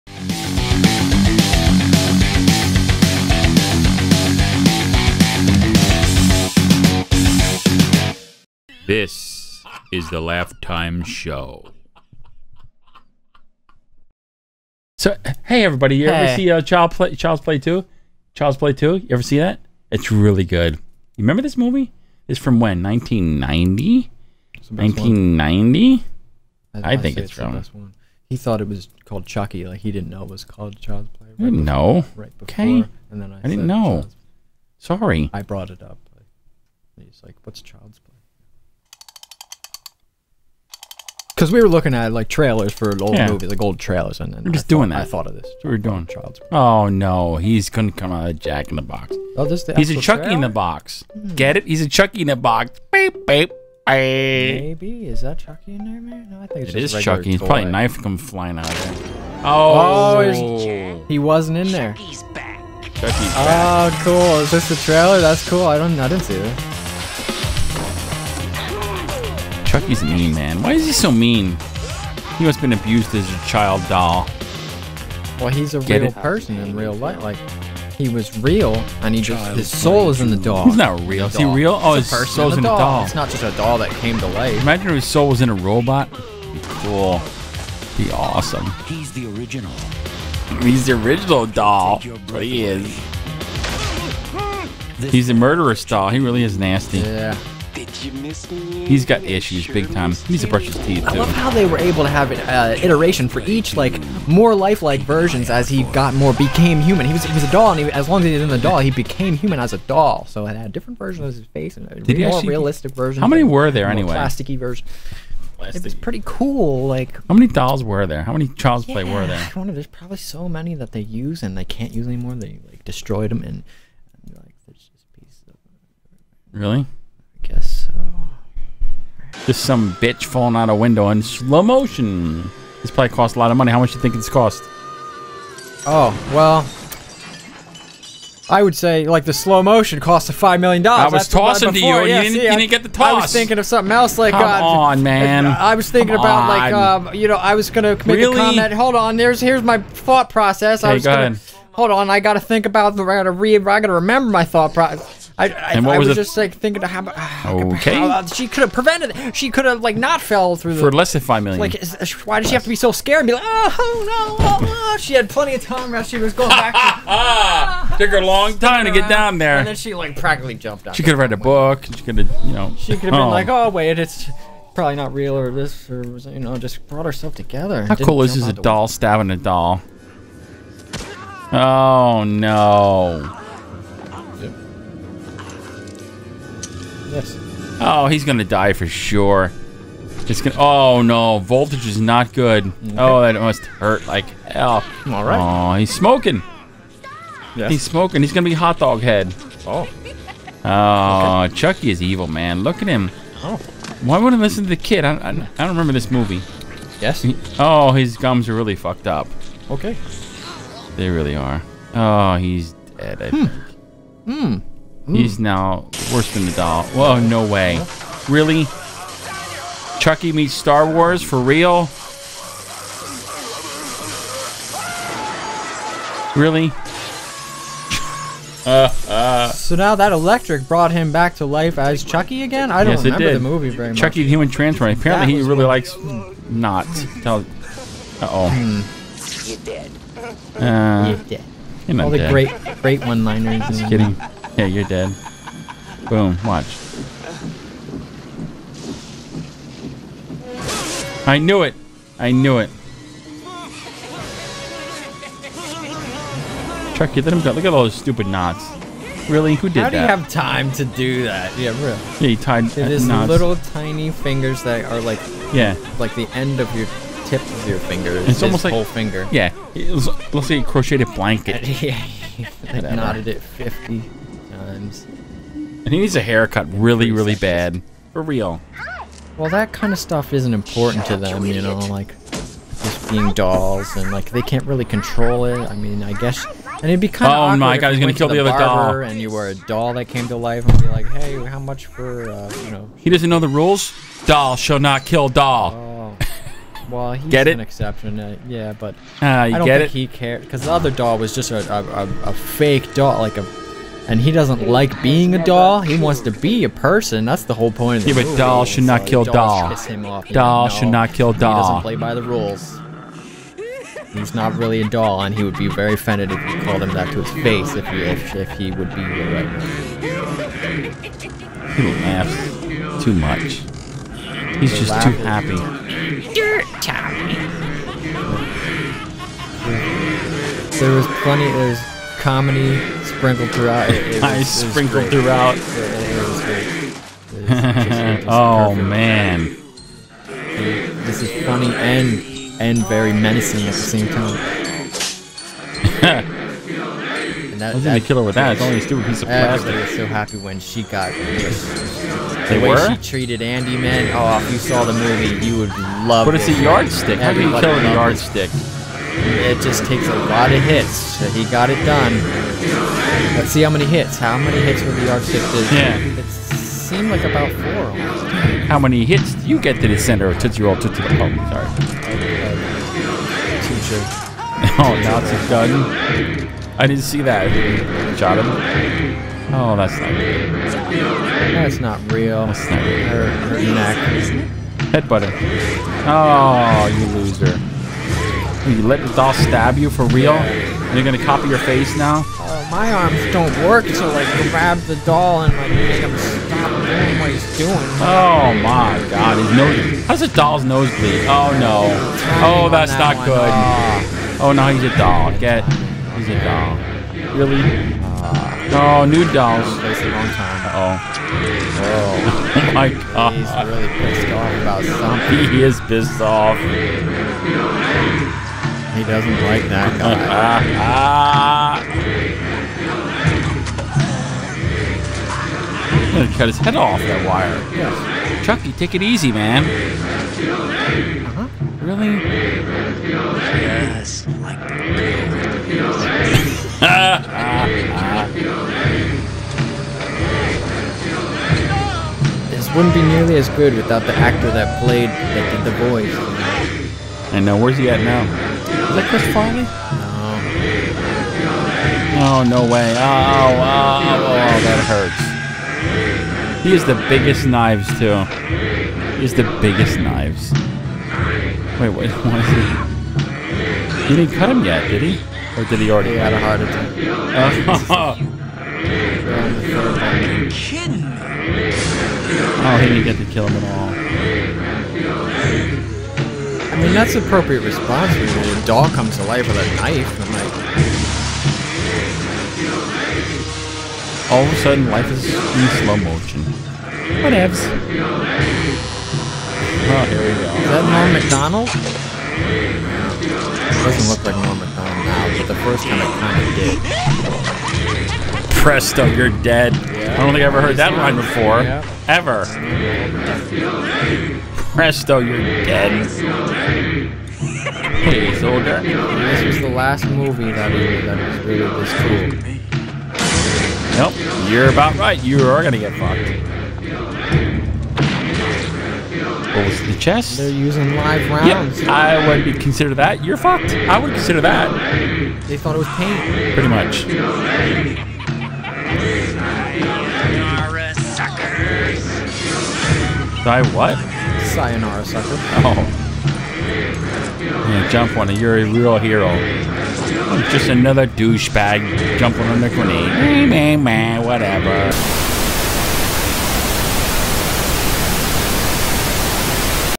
Child's Play Movie Review
We have fun making an ass out of the movie series "Child's Play".